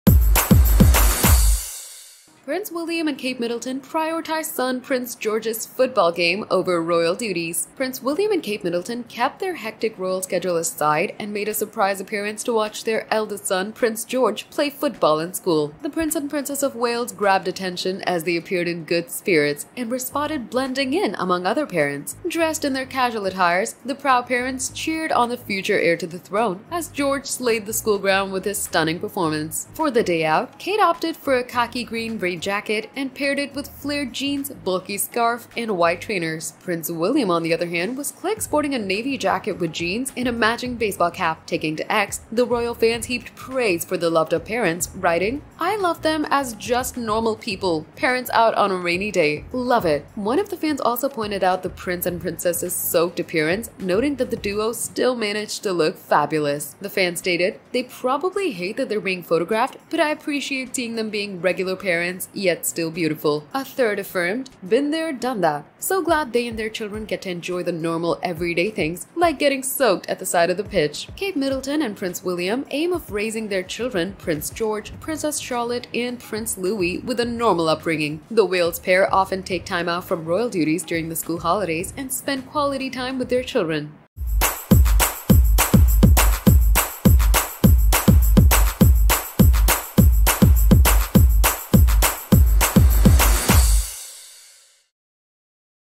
Prince William & Kate Middleton make their son proud! | Gossip Herald
Prince William and Kate Middleton cheered on Prince George as he played football on the school ground